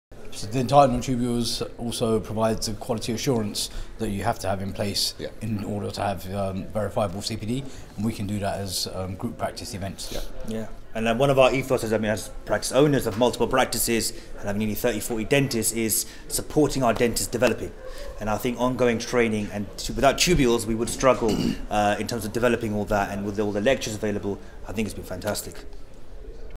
Sumair Khan and Jin Vaghela talks about their experience with dentinaltubules
Dr Jin Vaghela, Kish Patel, Sumair Khan and Ali Chohan are established dental practice owners and trainers based in the United Kingdom. As owners of the Smile Clinic Group and the Smile Academy, they have found Dentinaltubules absolutely valuable to their teams. here they explain how it has contributed to their groups success.